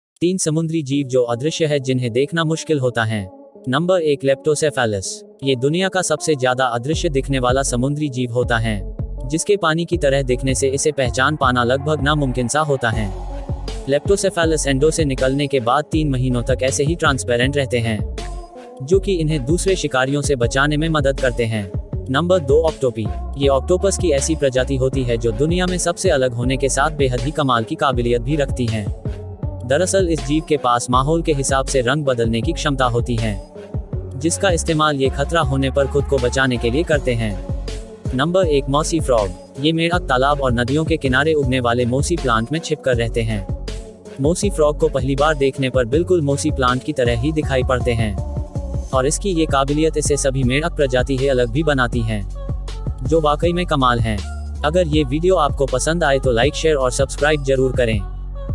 ऐसे समुंद्री जानवर जो गायब हो जाते हैं 🤯 | Sea Creatures | Mossy Frog | Octopus
ऐसे समुंद्री जानवर जो गायब हो जाते हैं 🤯 | सबसे अनोखे 3 समुंद्री जीव जो अदृश्य हैं  | Invisible & Transparent Sea Creatures Sea Creatures | Mossy Frog | Octopus